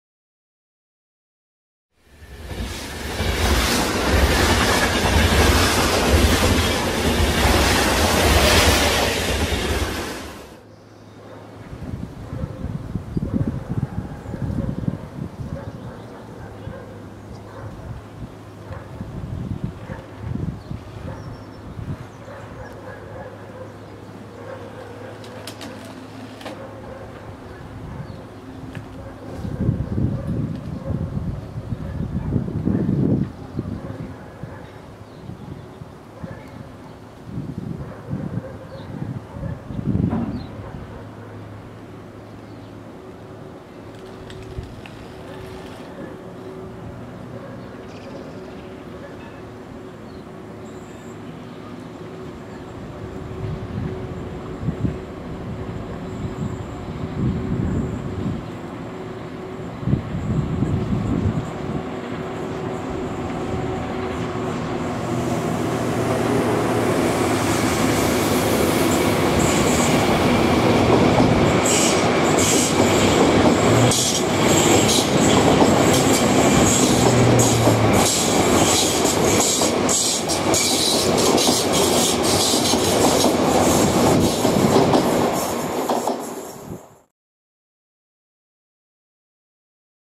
ETR 400 in transito in Linea Lenta a Monterotondo - ETR 400 running on the Slow Line in Monterotondo
Un ETR 400 è in lenta corsa nei pressi della stazione di Monterotondo.

An ETR 400 is running slowly near the station of Monterotondo.